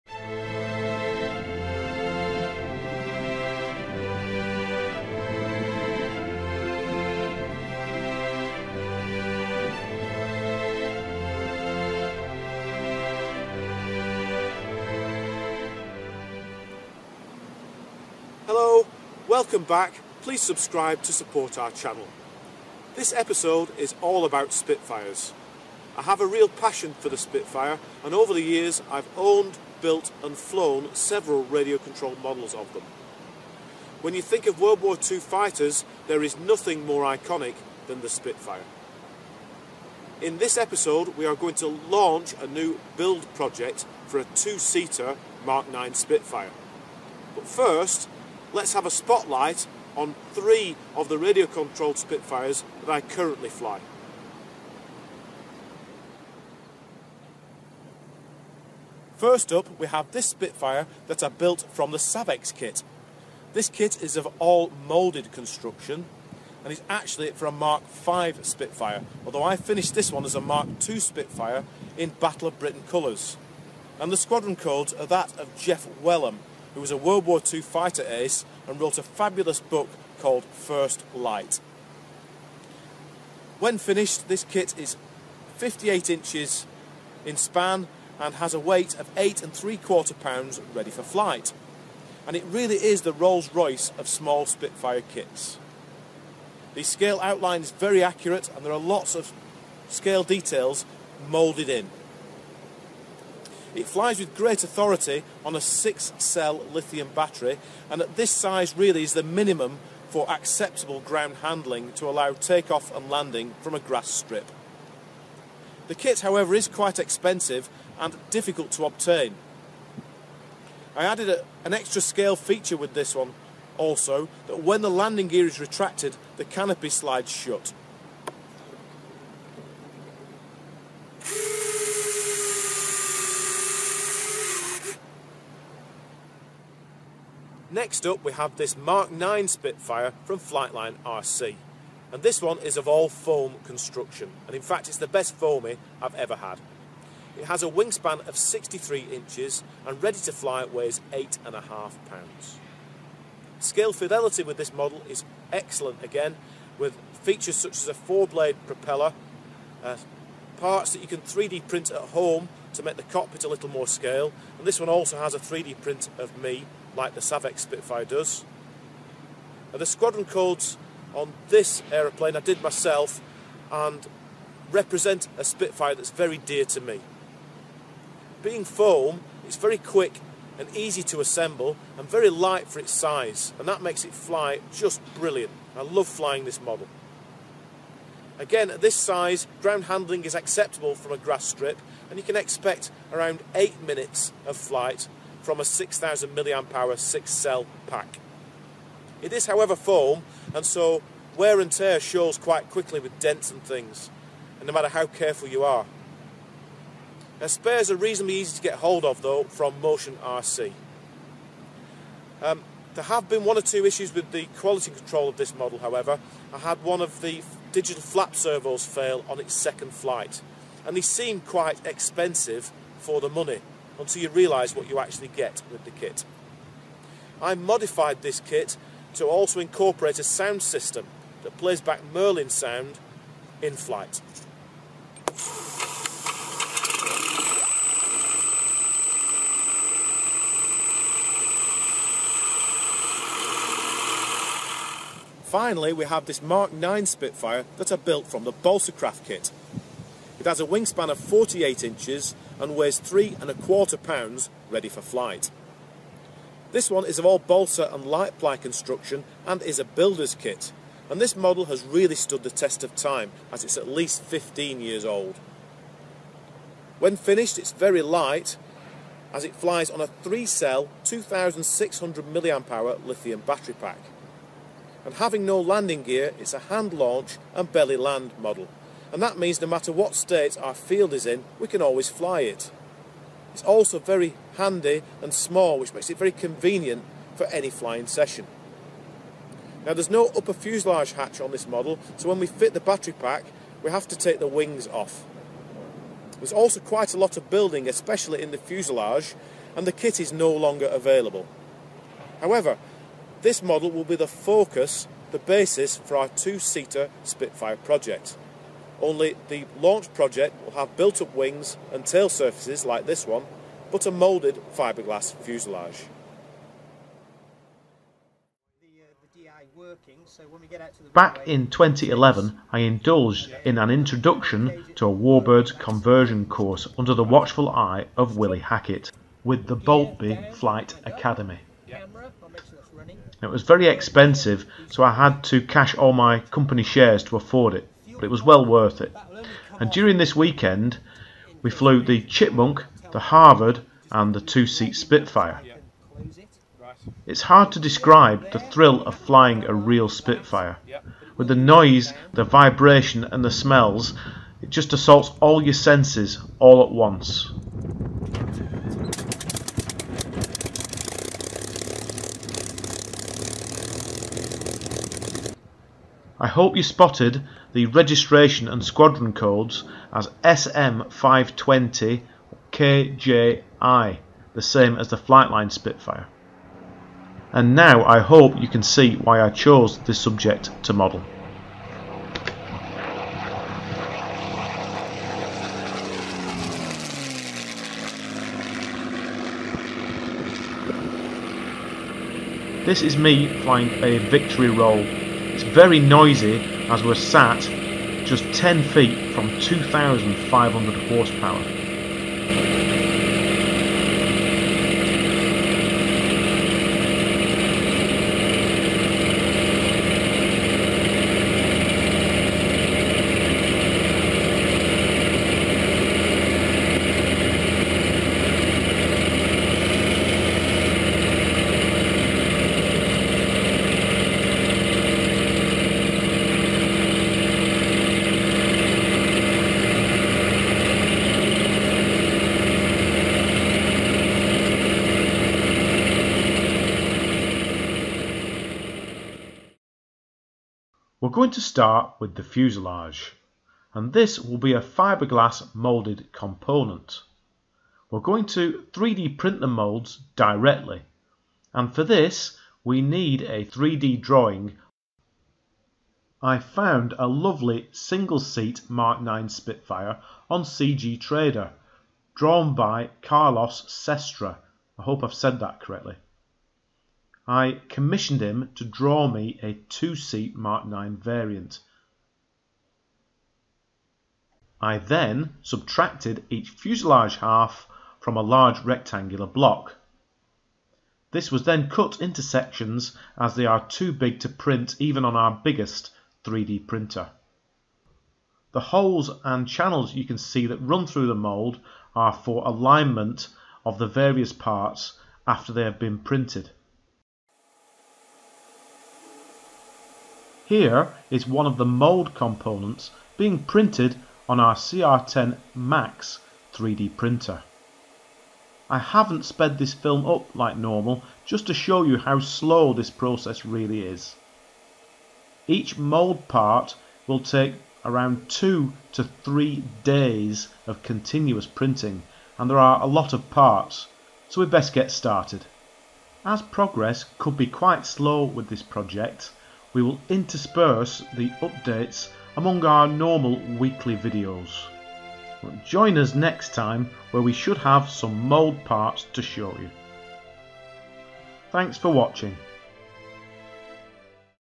Scratch Built RC Spitfire TR9 - Part 1
Building completely from scratch ! A radio controlled (RC) 2 seat Spitfire MkTR9.  In this video we launch the project, and take a closer look at the proposed design.

We take an in-depth look at 3 other RC Spitfires, and lay out the features of the new build project, involving; 3D printing, composite moulding, laser cutting and traditional building techniques.

Links;